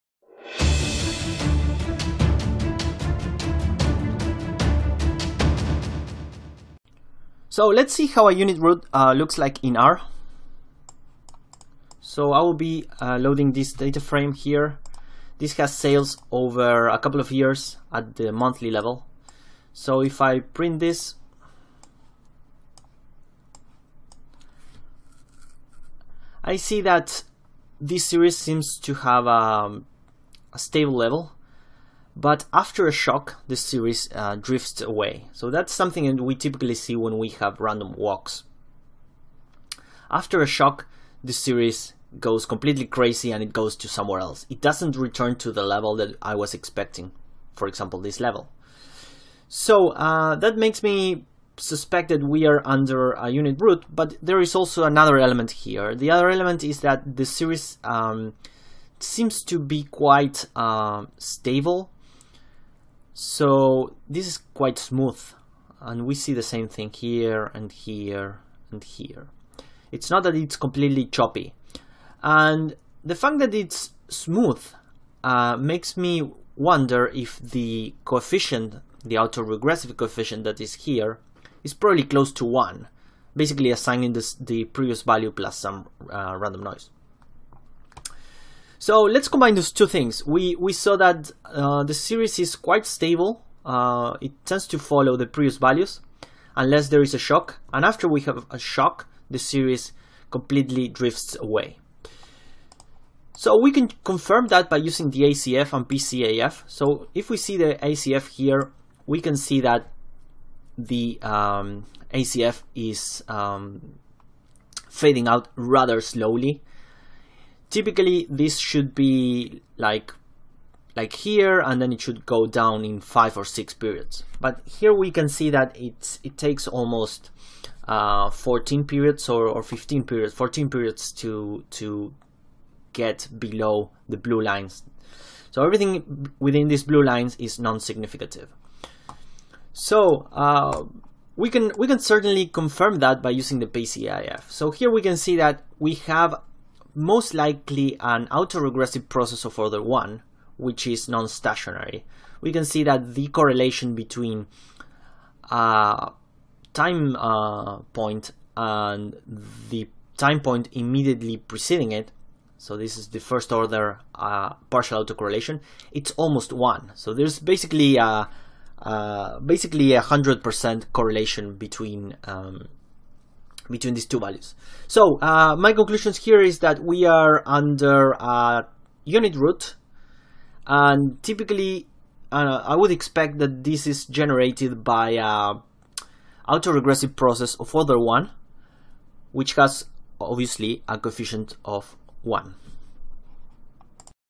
Identifying a random walk time series in R
Random walk processes tend to drift away, and do not have a well defined "mean". We use the autocorrelation and partial autocorrelation function to identify them